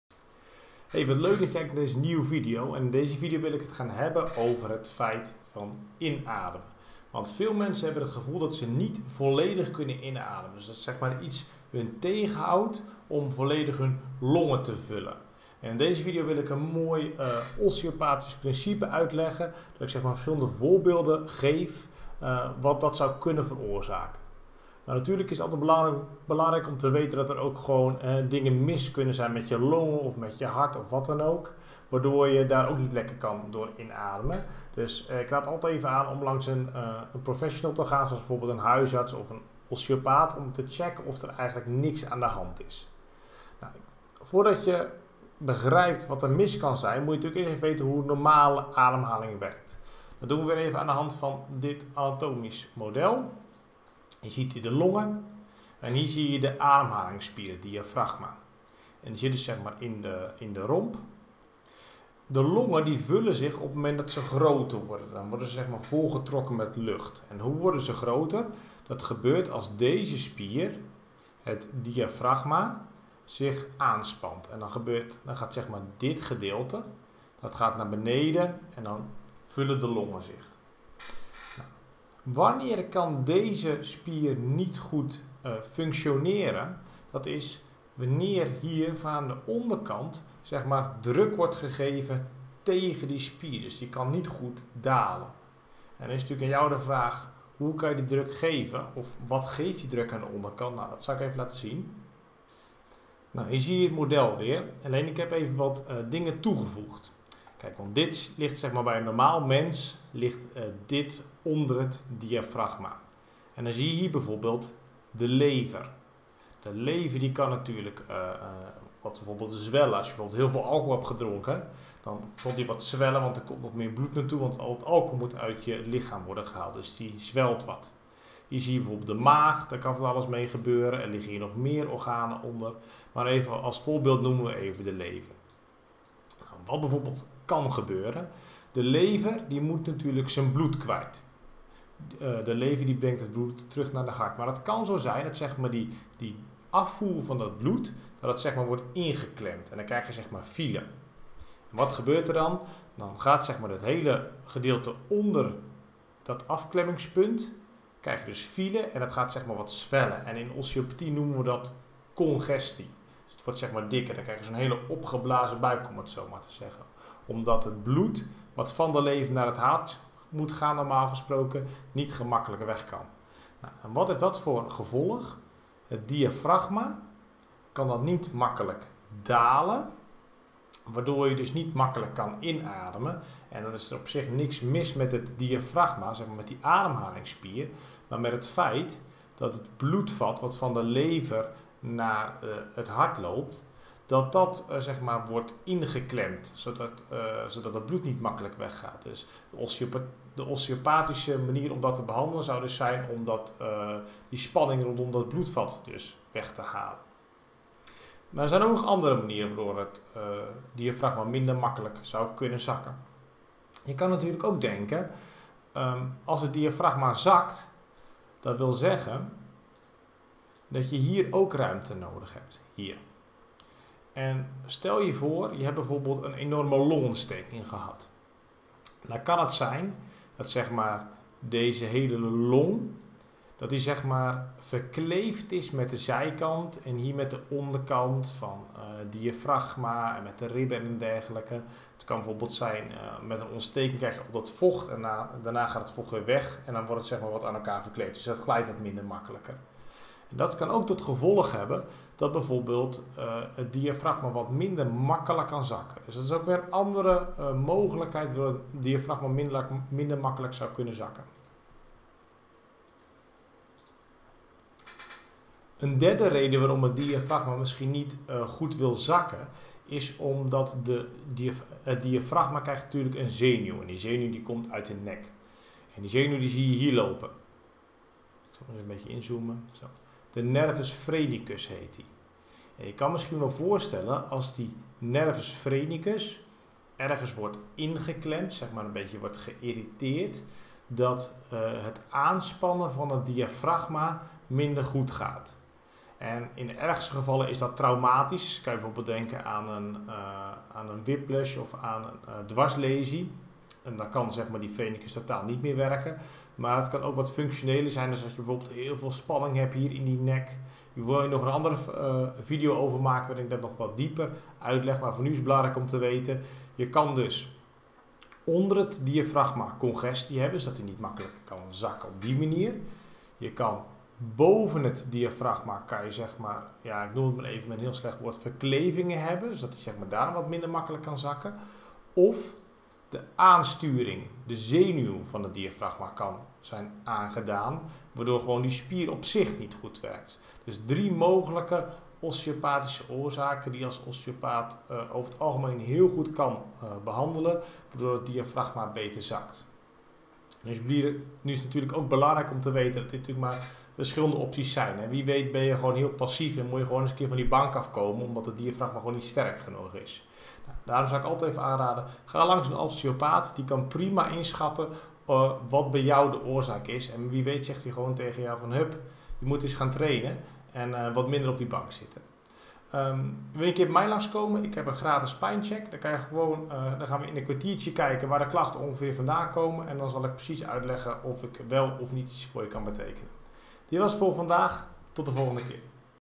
Niet voldoende lucht? Of kan je niet goed inademen? Osteopaat legt mogelijke oorzaken uit.
Heb jij moeite met diep ademhalen of het gevoel dat je niet genoeg lucht krijgt? 😤 Dit kan te maken hebben met blokkades in je ribben, nek, of zelfs je buikorganen. In deze video leg ik als osteopaat uit hoe het lichaam samenwerkt voor een goede ademhaling en hoe osteopathie je kan helpen om weer makkelijk en diep te ademen. 🌬️ Waarom wachten? Maak het jezelf gemakkelijk en boek direct een afspraak